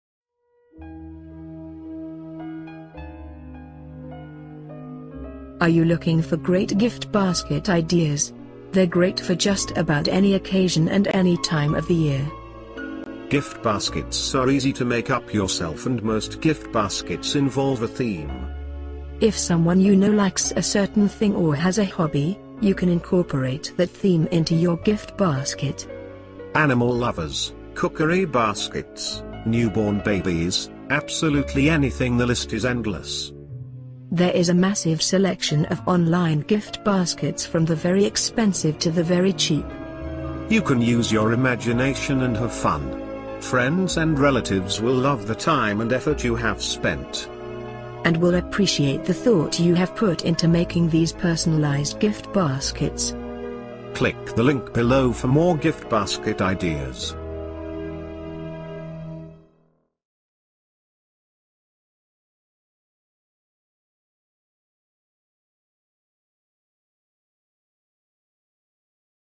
Gift basket ideas
Everyone loves to receive a gift basket. They're great for just about any occasion, corporate gift baskets are often given by businesses to their customers as a token of their appreciation.